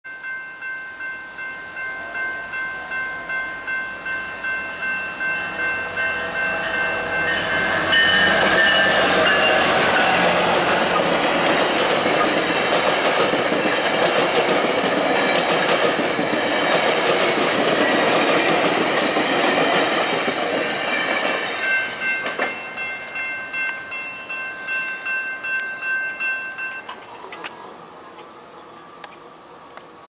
MP40 #611 on an Eastbound GO Train
An eastbound GO train is seen crossing Stavebank Ave. in Mississauga and approaching Port Credit GO station with MP40PH-3C and and an eight car consist.  Cabcar #354 brings up the rear.